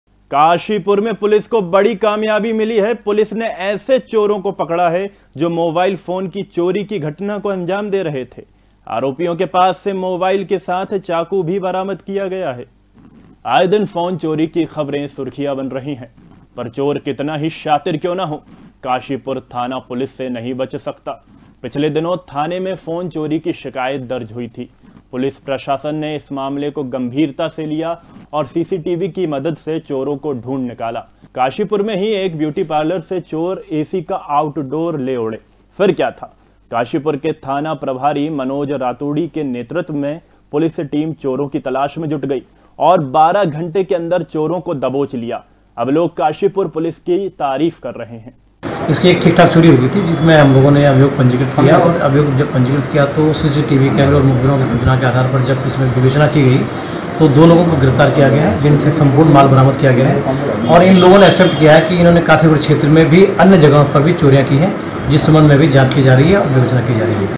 DAKHAL NEWS काशीपुर में पकड़े गए मोबाइल चोर
काशीपुर में पकड़े गए मोबाइल चोर
पुलिस को मिली बड़ी कामयाबी

news
daily news
today news
dakhal news
crime
crime updates
news update